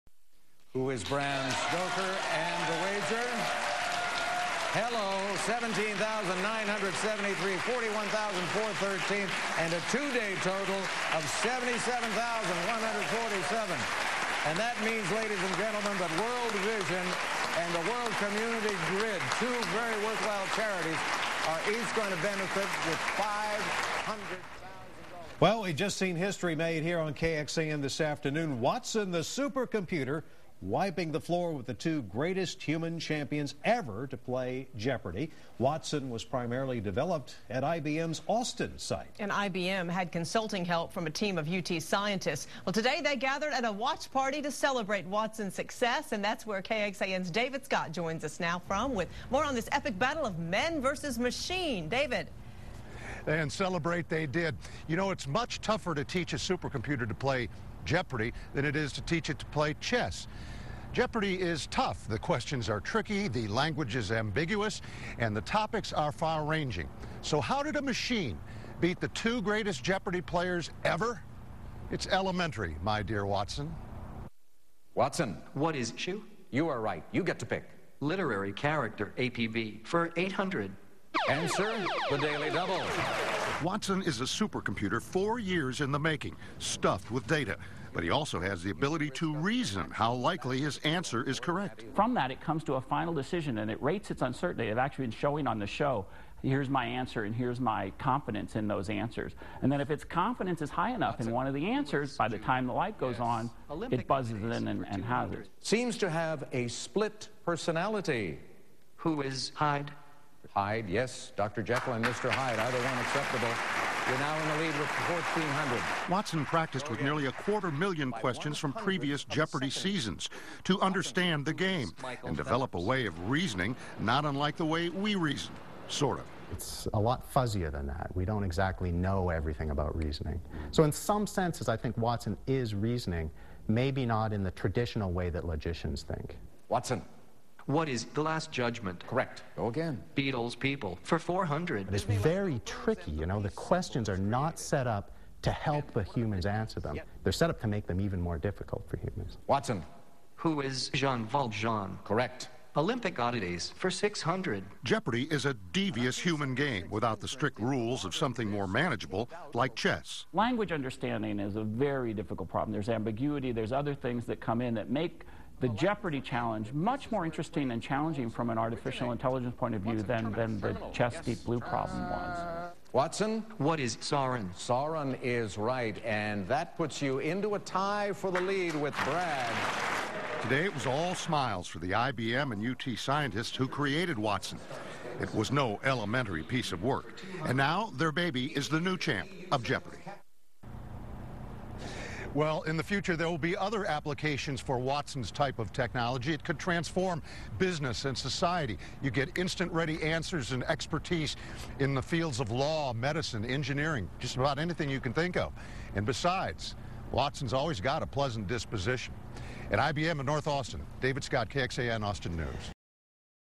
'Watson' computer wins at 'Jeopardy'
Watson the super computer has defeated the two greatest human champs in the history of "Jeopardy."